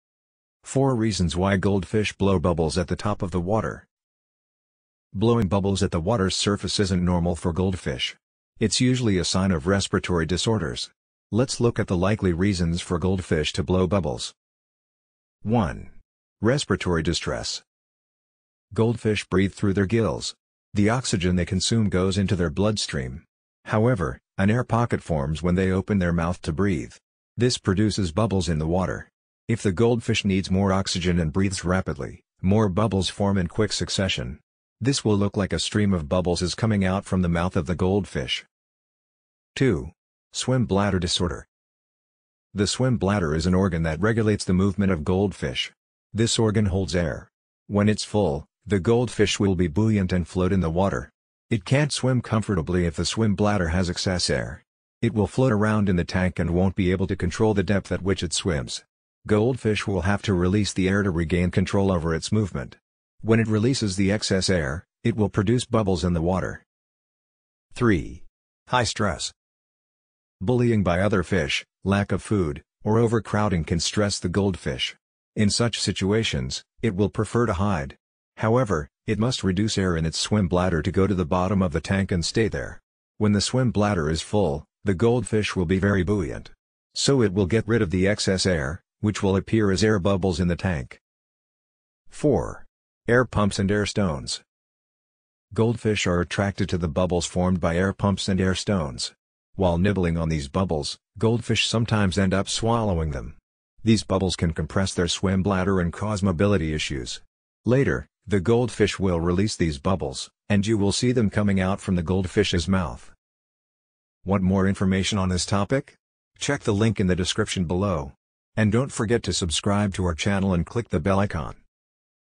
4 Reasons Why Goldfish Blow Bubbles at The Top of The Water
4 Reasons Why Goldfish Blow Bubbles at The Top of The Water
In this video, we will go over the reasons why goldfish swim at the top of the water.